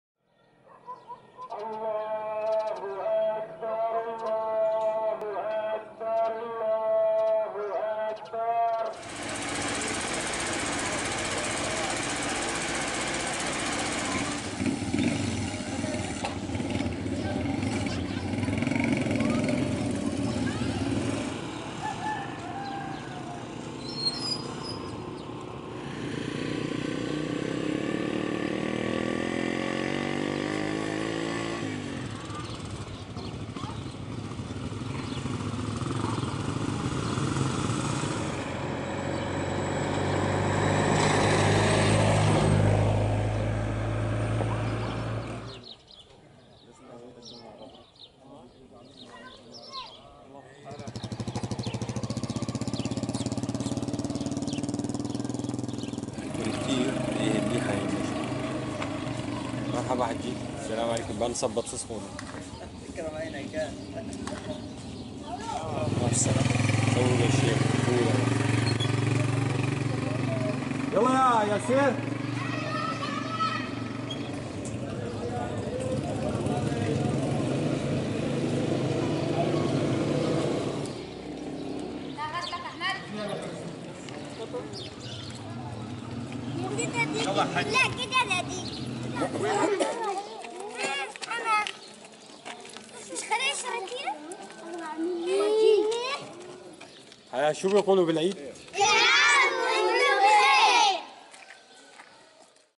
Hama: Eid Al-Fitr Prayers In Talf Town 25-6-2017
People in Talf town southern Hama province welcomed Eid Al-Fitr by performing its prayers, later they headed to the graves to visit their dead relatives and friends, despite of what the area witnesses from de-escalation in the cessation of hostilities and shelling with artillery as it is included in the four de-escalation zones agreement signed in Astana 4.